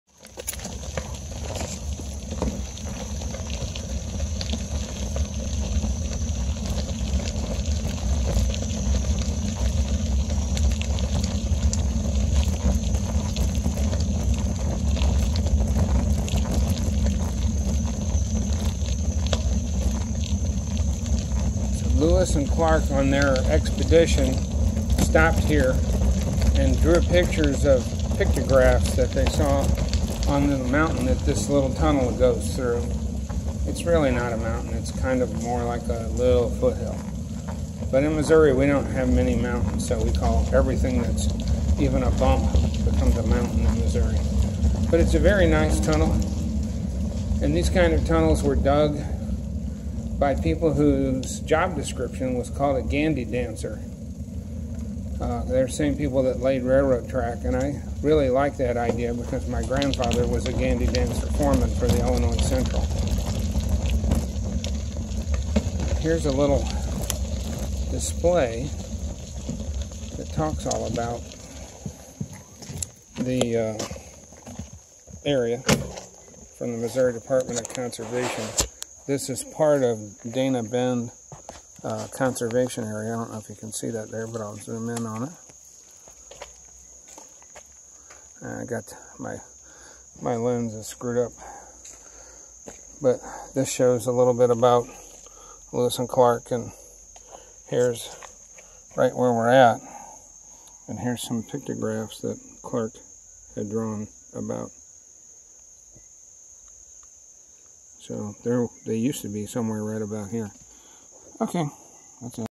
Rochport tunnel KATY Trail Lewis and Clark
Eastbound view of the KATY Trail. Gandy Dancer built tunnel. My grandfather was an Gandy Dancer foreman on the Illinois Central line.